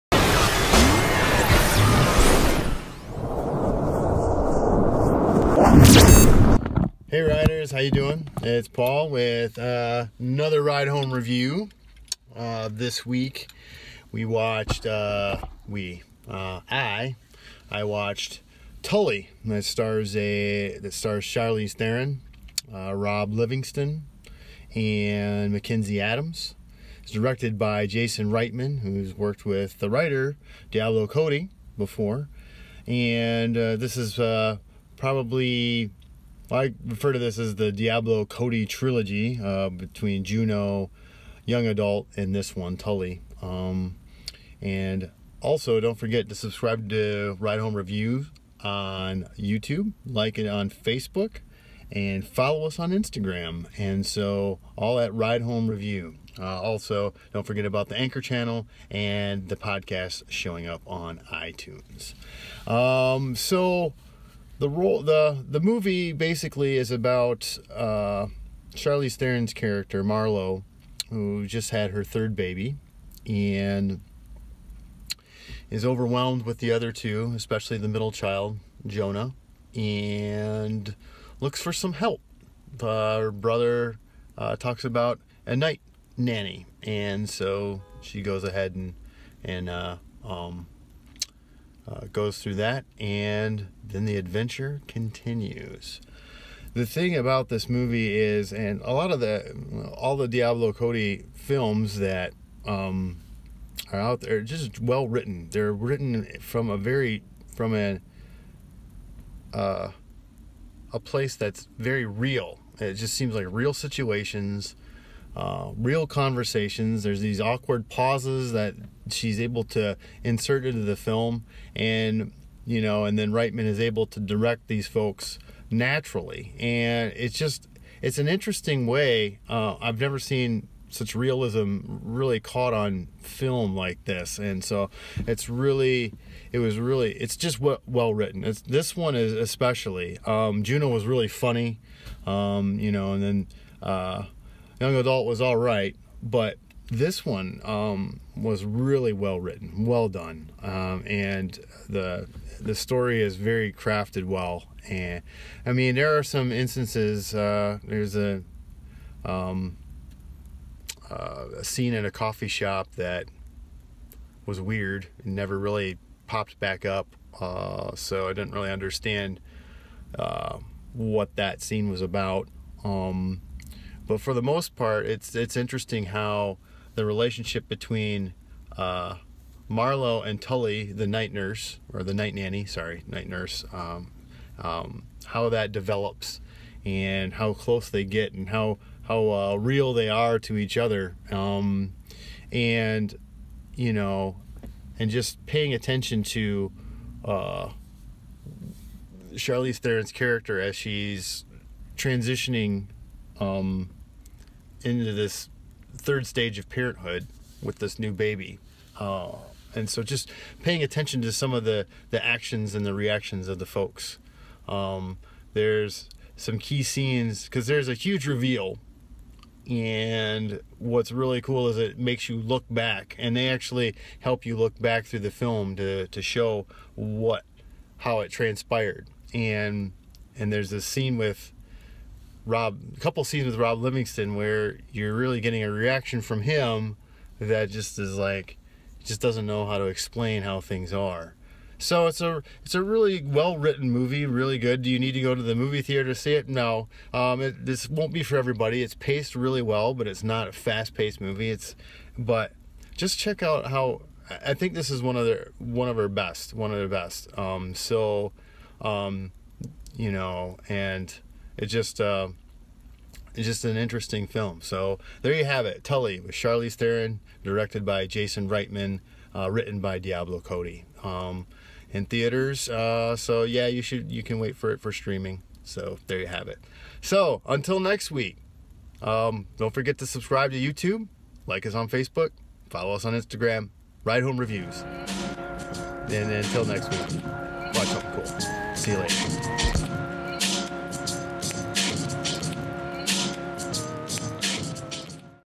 RideHomeReview - Tully
Synopsis: A mother of three hires a night nanny to help with her newborn.

Director: Jason Reitman
Writer: Diablo Cody
Stars: Charlize Theron, Mackenzie Davis, Ron Livingston

What I liked: The writing, the acting, the directing

What I didn't like: Some non-connected scenes

What to watch for: The big reveal